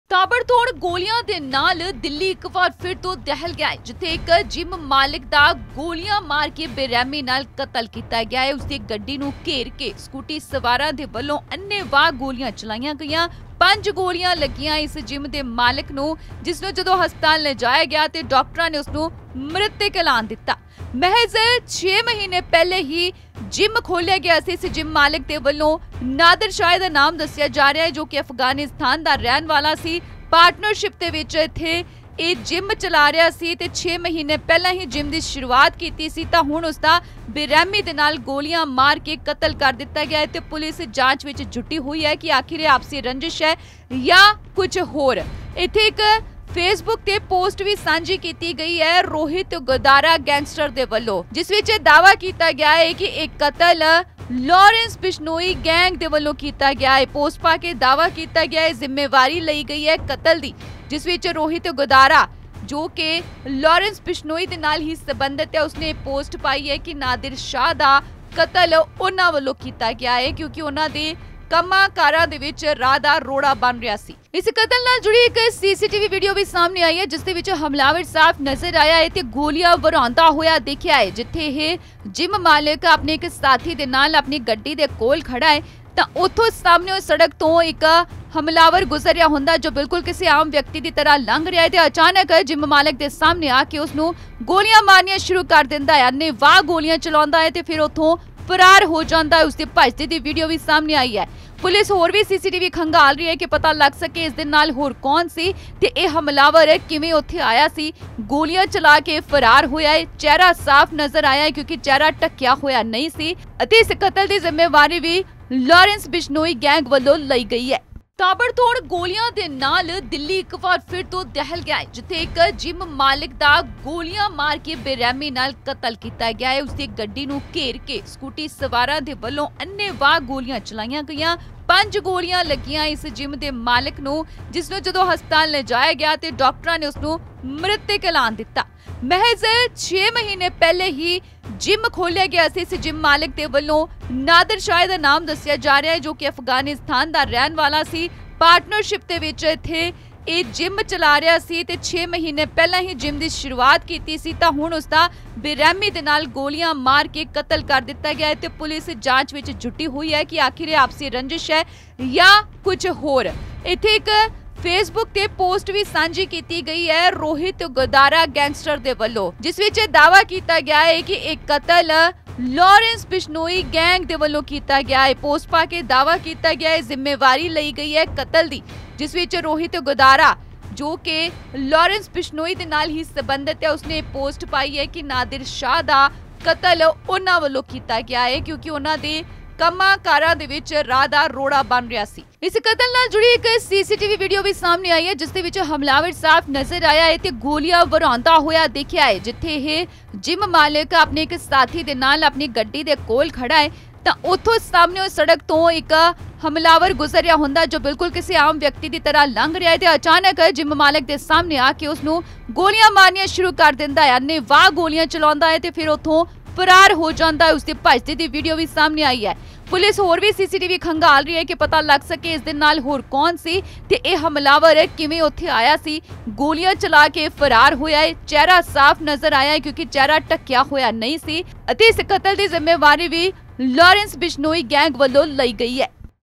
Delhi Murder CCTV | ਜਿਮ ਮਾਲਕ ਦਾ ਗੋਲੀਆਂ ਮਾਰ ਕੇ ਕਤਲ, ਵੱਡੀ ਗੈਂਗਵਾਰ ! Firing | N18V
ਜਿੰਮ ਮਾਲਕ ਦਾ ਗੋਲੀਆਂ ਮਾਰ ਕੇ ਕਤਲ! ਬਾਹਰ ਖੜ੍ਹੇ 'ਤੇ ਹਮਲਾਵਰ ਨੇ ਚਲਾਈਆਂ ਤਾਬੜ ਤੋੜ ਗੋਲੀਆਂ,  ਗੈਂਗਸਟਰ ਨੇ ਫੇਸਬੁੱਕ 'ਤੇ ਪੋਸਟ ਪਾ ਲਈ ਕਤਲ ਦੀ ਜ਼ਿੰਮੇਵਾਰੀ, ਸੀਸੀਟੀਵੀ 'ਚ ਦਿਖਿਆ ਹਮਲਾਵਰ